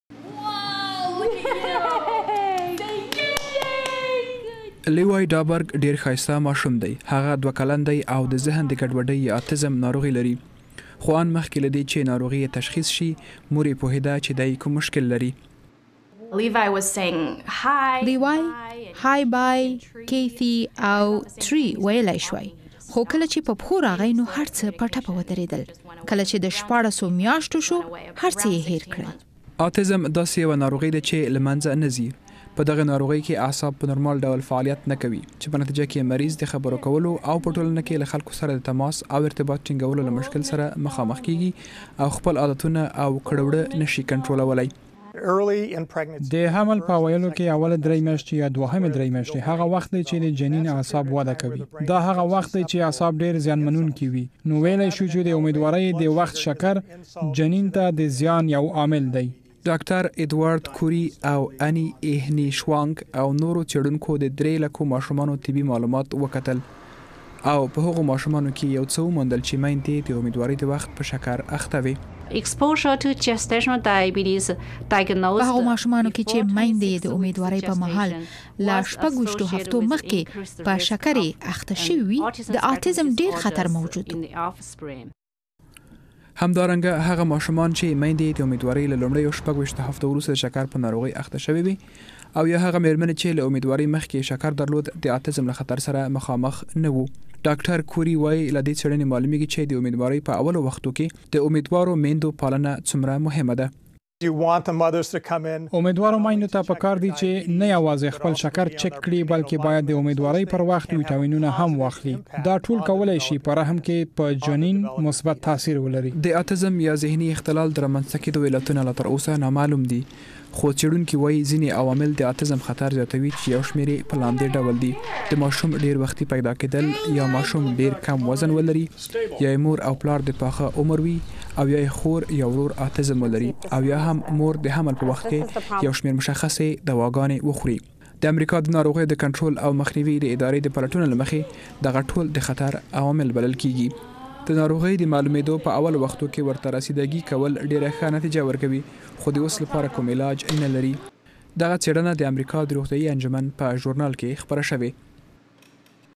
آیا د آتیزم د ناروغۍ په اړه پوره معلومات لرئ؟
کله چې یوه میندواره میرمن د امیدوارۍ په لومړیو ۲۶ هفتو کې په شکر اخته شي، د یوې څېړنې له مخې ماشوم یې د ذهن د ګډوډۍ یا آتیزم په ناروغۍ د اخته کیدو له زیات خطر سره مخامخ کیدای شي.